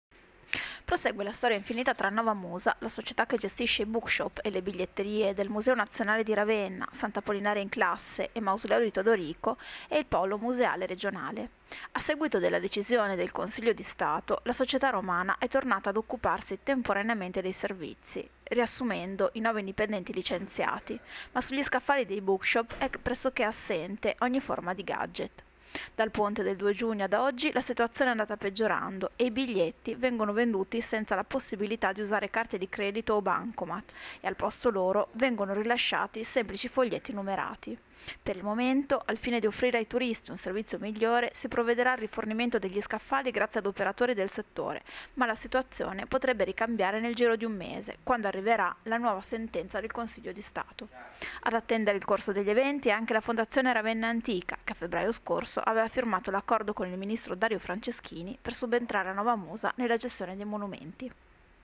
RAVENNA : Novamusa riassume i dipendenti licenziati
Prosegue la storia infinita tra Novamusa, società che gestisce i bookshop e le biglietterie di Museo nazionale di Ravenna, Sant’Apollinare in Classe e Mausoleo di Teodorico e il polo museale regionale. A seguito della decisione del Consiglio di stato la società romana è tornata ad occuparsi temporaneamente dei servizi, riassumendo i nove dipendenti licenziati, ma sugli scaffali dei bookshop è pressochè assente ogni forma di gadget. Dal ponte del 2 giugno ad oggi la situazione è andata peggiorando e i biglietti vengono venduti senza la possibilità di usare carte di credito o bancomat, e al posto loro vengono rilasciati semplici foglietti numerati. Per il momento, al fine di offrire ai turisti un servizio migliore, si provvederà al rifornimento degli scaffali grazie ad operatori del settore, ma la situazione potrebbe ricambiare nel giro di un mese, quando arriverà la nuova sentenza del consiglio di stato. Ad attendere il corso degli eventi è anche la Fondazione RavennAntica, che a febbraio scorso aveva firmato l’accordo con il ministro Dario Franceschini per subentrare a Novamusa nella gestione dei monumenti.